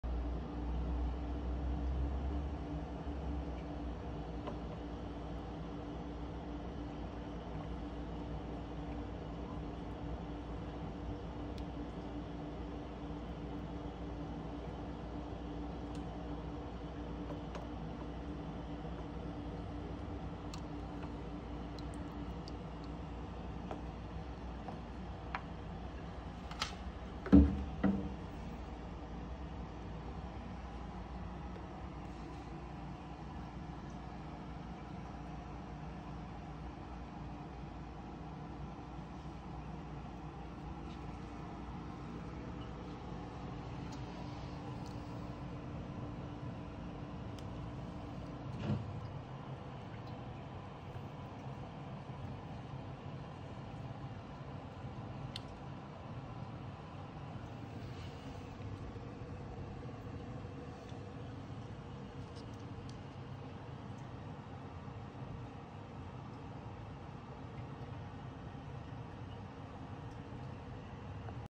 Wall Hammer Losing Flesh
More issues with my euphilias not sure what to do. They are open and puffy but they suddenly decided to eject the heads.